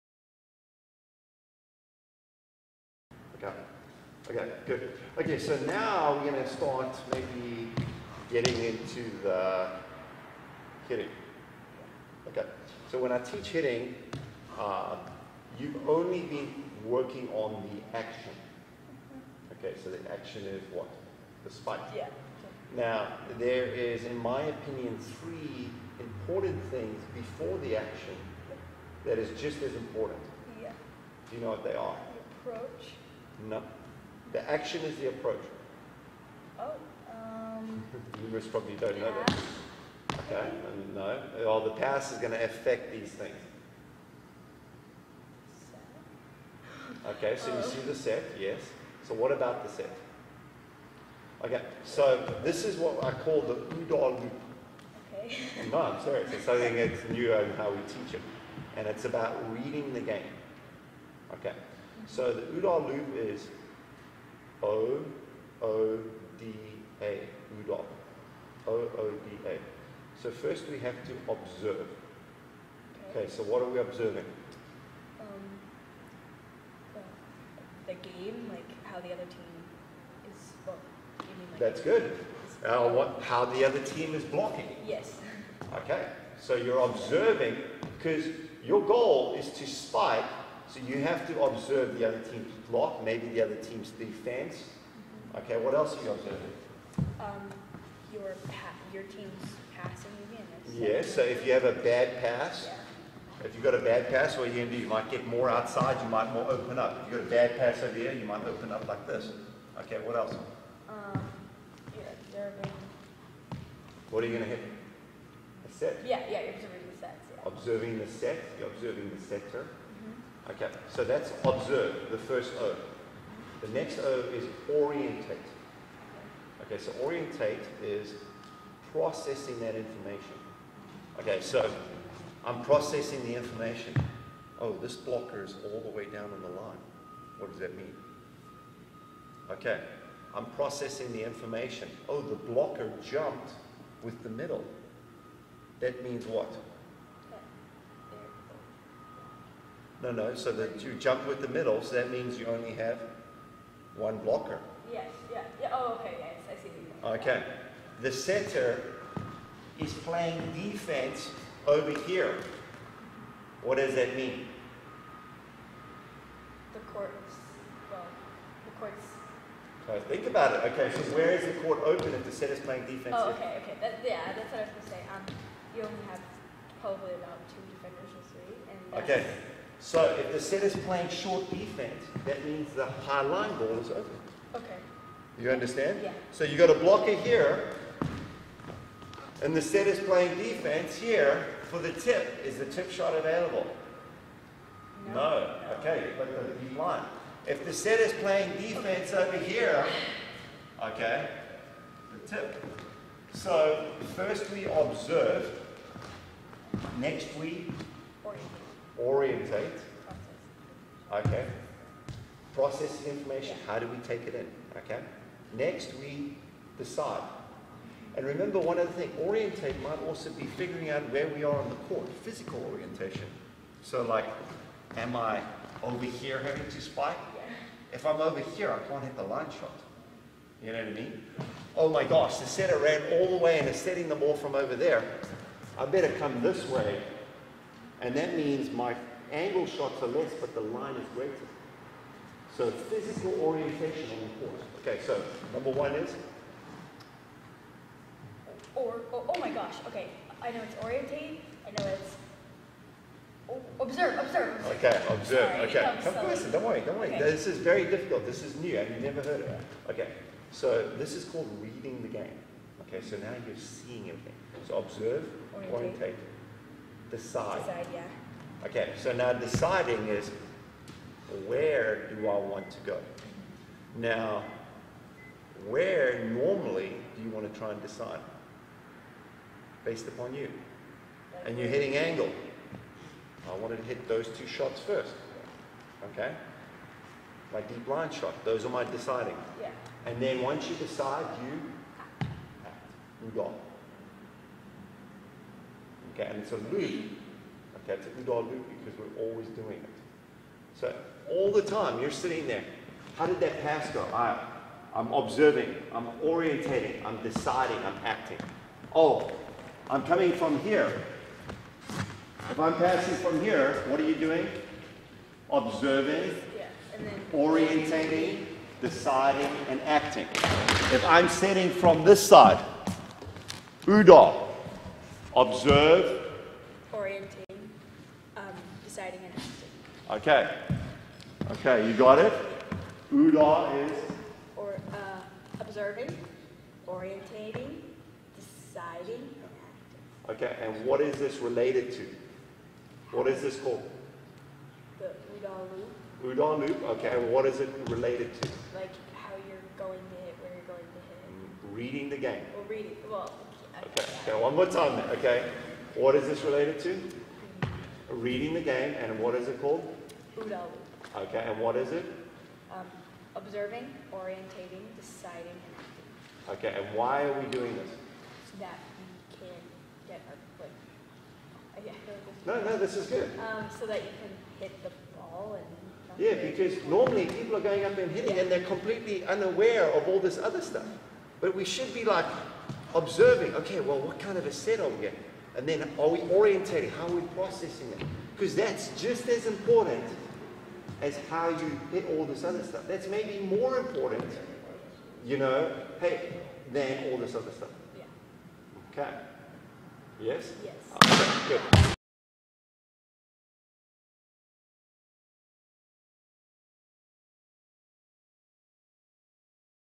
How To Coach the OODA Loop Verbal Cues To Youth Players - OODA Loop For Volleyball Coaching
This video contains a example and verbal cues of how Andor Gyulai teaches volleyball spiking technique using the OODA Loop. The OODA Loop’s founded by John Boyd (the fighter pilot who changed modern warfare), is a system that could be used to help improve human reaction speed as we respond to stimuli.

As volleyball coach I believe the same proven principals of OODA Loop can be successfully used to help players improve how they respond to stimuli on the volleyball court. This includes using the OODA Loop Coaching System for “Reading the Game” and Volleyball Strategy.

See me videos and articles on coaching the OODA Loop for volleyball.